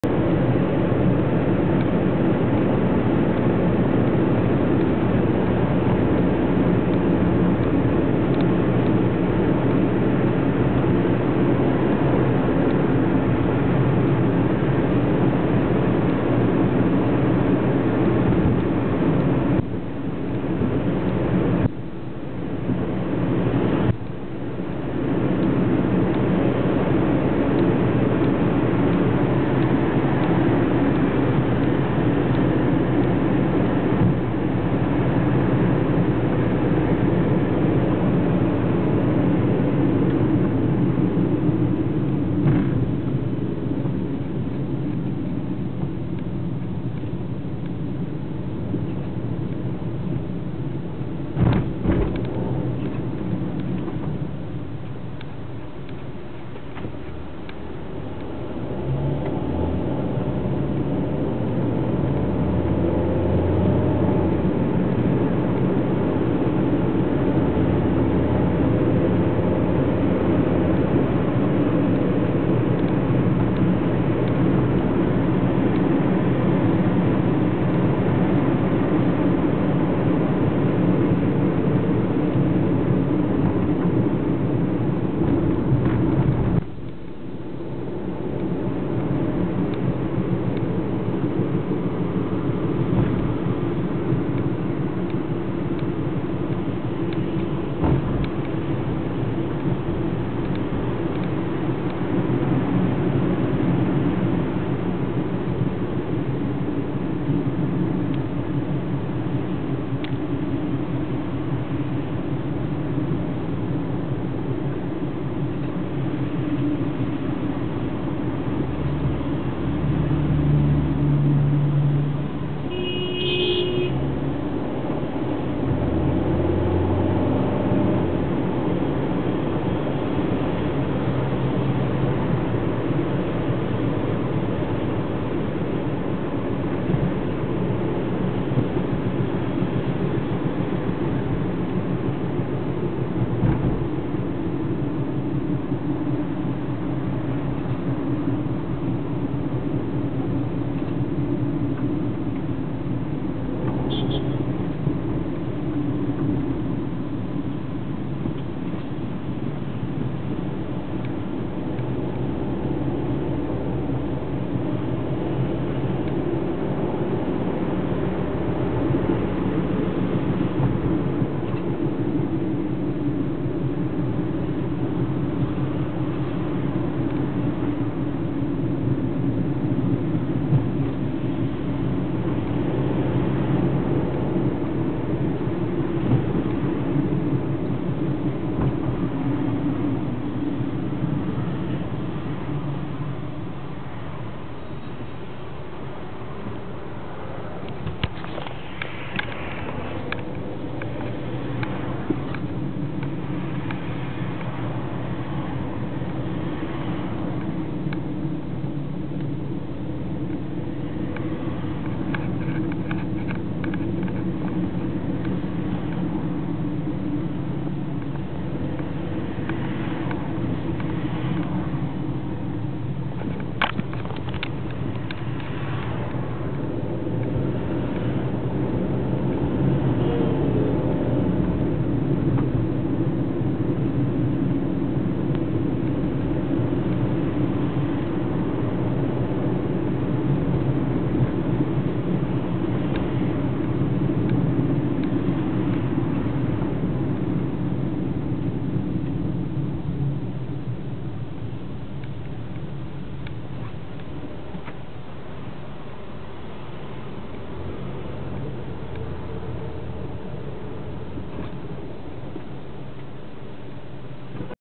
Small tour in Tripoli جولة في طرابلس الغرب-ليبيا
جولة في طرابلس من كوبري المصري إلي كوبري طريق السكة بتاريخ 8-1-2011 الساعة 11:30